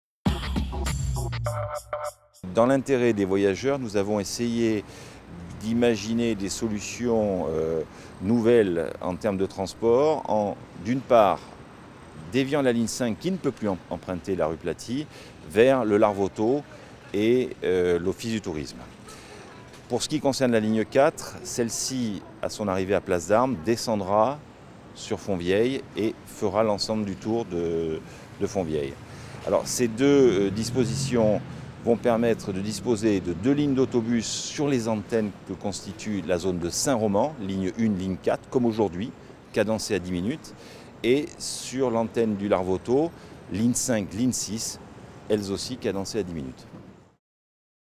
Modifications du Réseau de la Compagnie des Autobus de Monaco
Roland de Rechniewski, Directeur d'Exploitation de la Compagnie des Autobus de Monaco nous explique les changements qui interviendront sur les lignes des Autobus de Monaco à compter du Lundi 5 janvier 2015 et notamment les modifications sur la Ligne 5 et la Ligne 4 suite à la fermeture d'une partie de la rue Plati.